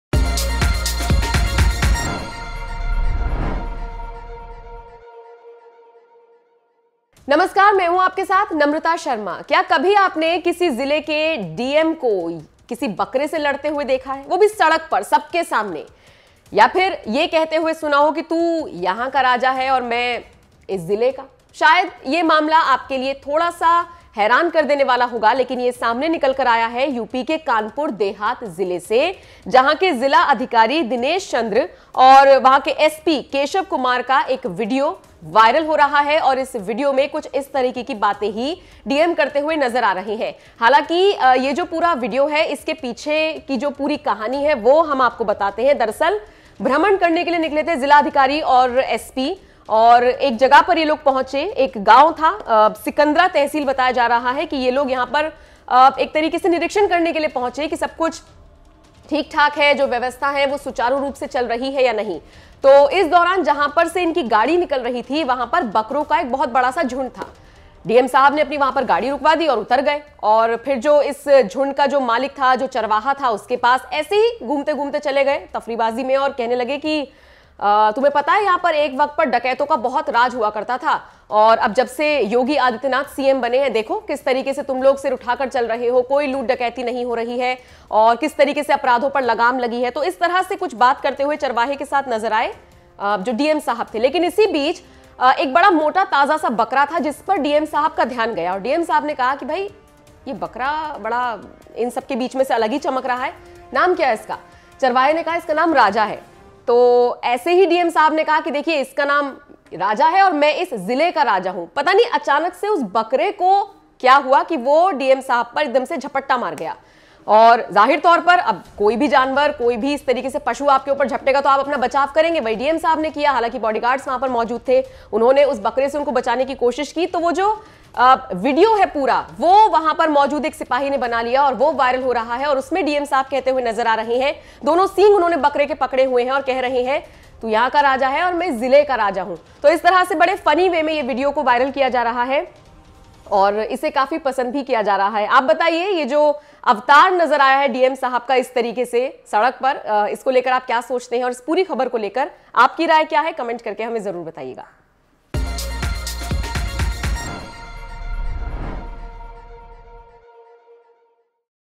कानपुर के DM का बकरे से भिड़ते हुए वायरल हुआ वीडियो, बोले- तू राजा तो मैं भी राजा
कानपुर से एक बेहद दिलचस्प मामला निकल कर सामने आया है। कानपुर देहात डीएम भ्रमण करने के लिए निकले और रास्ते में नजर आए बकरे से भिड़ गए।